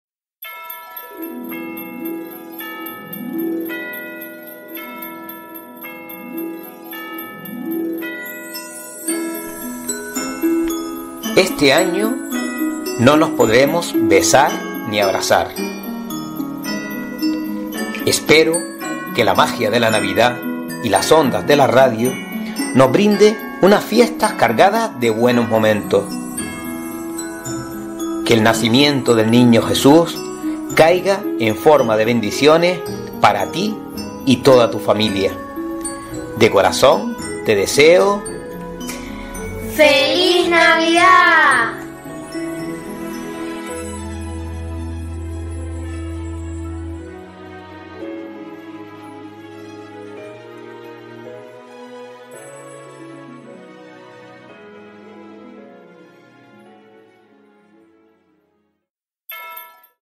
FELIZ NAVIDAD
Te deseo que pases una FELIZ y radiofónica NAVIDAD. Que las ondas de la radio inunden de paz y alegría estos días festivos. Comparto contigo mi podcast navideño.😘😘